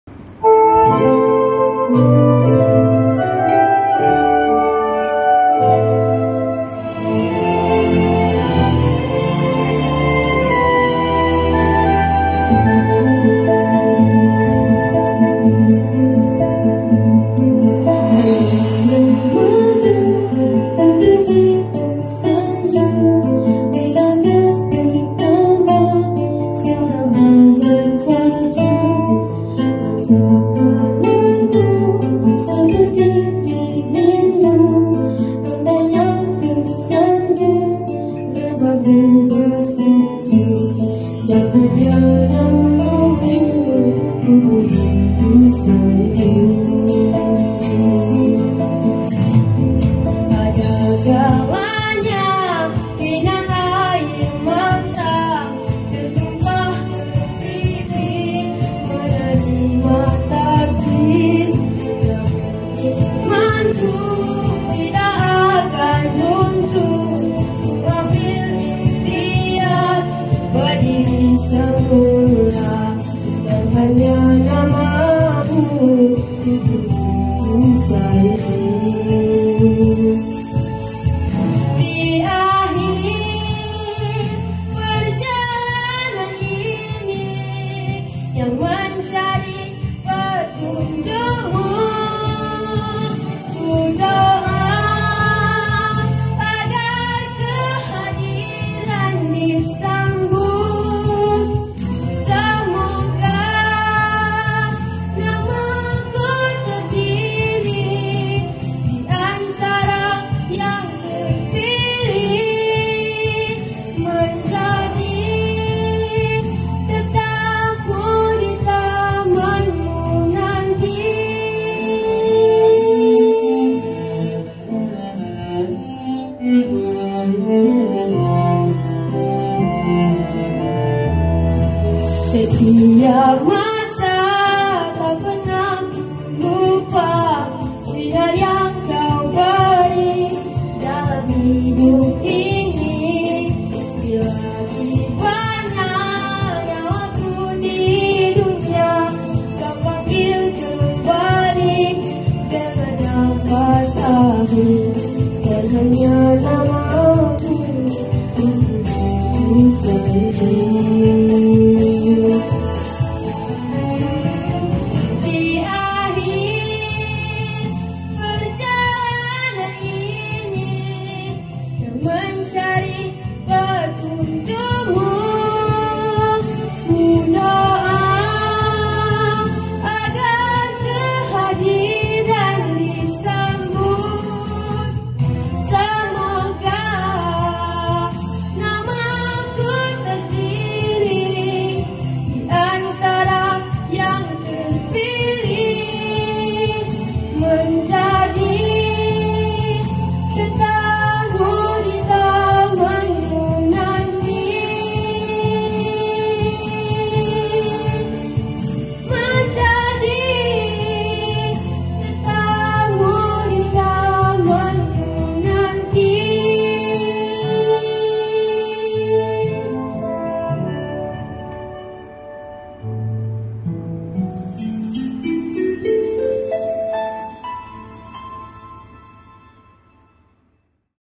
e-PERTANDINGAN NYANYIAN SOLO LAGU KEROHANIAN SEKOLAH RENDAH (L) | DAERAH GOMBAK
NAMA PESERTA : MUHAMMAD ARIF RAFIQQIN BIN ABDULLAH
KATEGORI : LELAKI (SEKOLAH RENDAH)